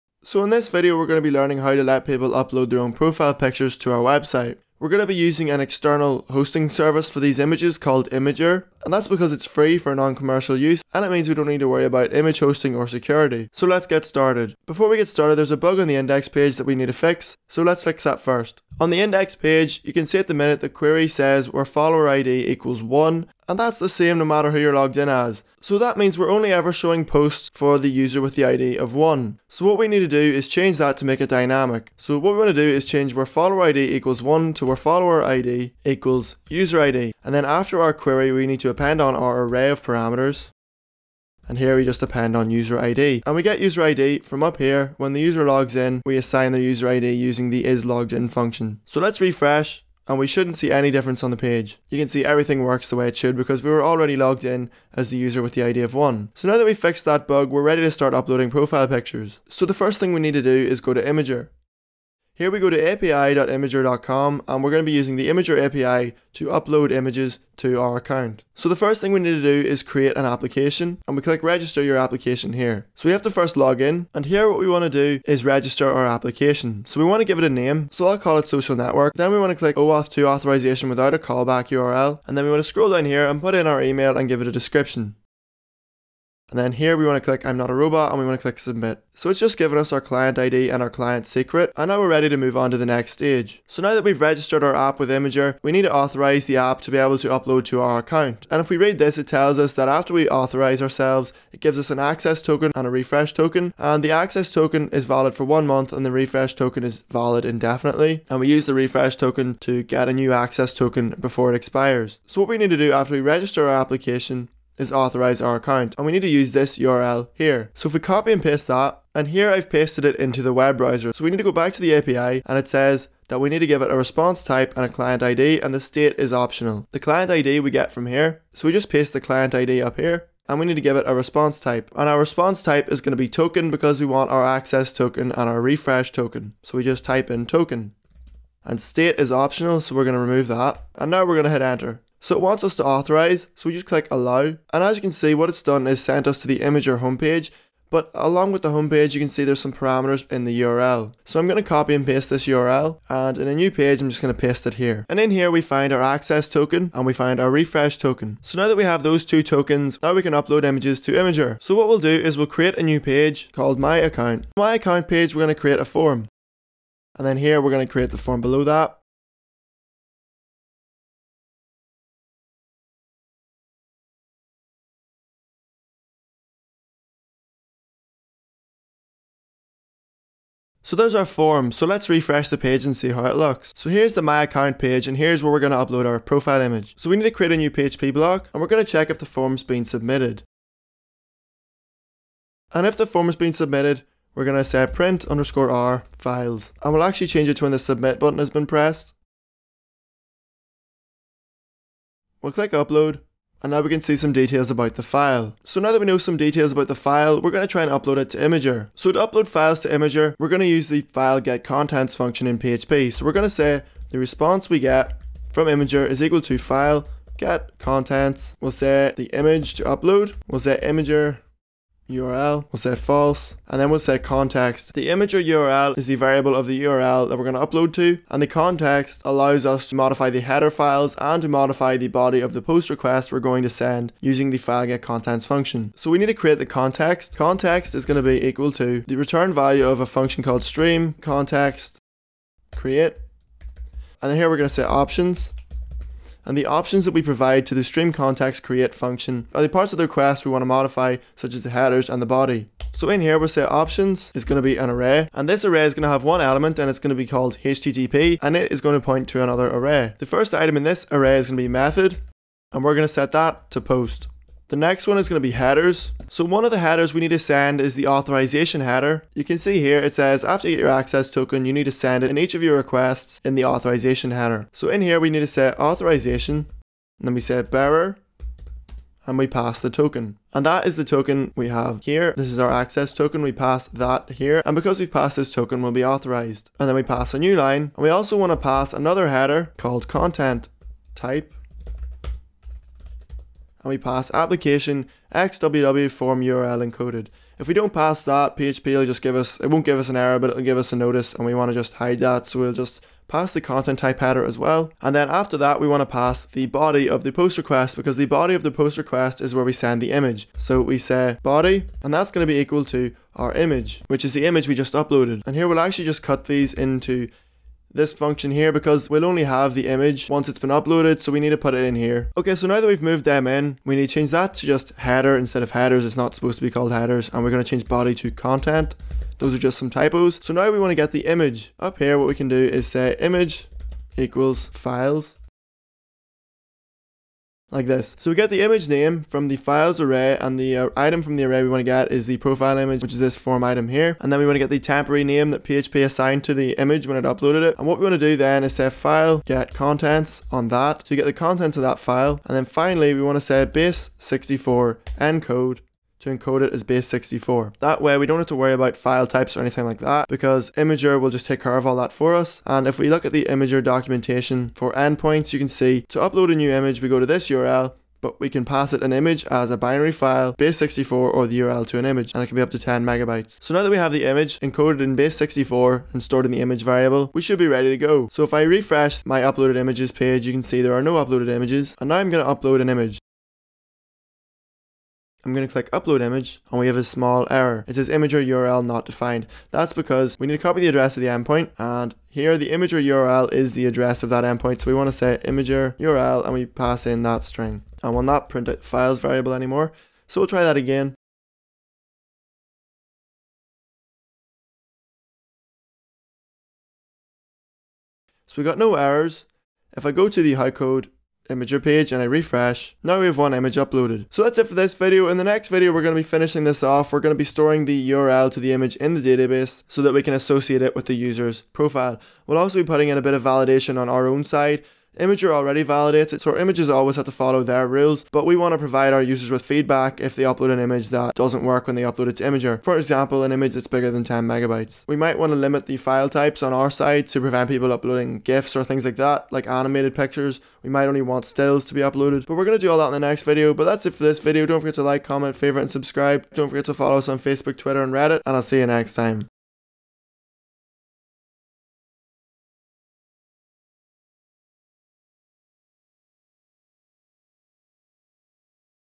New Social Network - Part 20 - Profile Images with Imgur
In this video we'll be learning how to use Imgur as an image hosting service to host the images on our site, taking care of all of the security problems for us.

We'll also be fixing a bug with the timeline feature on the index page.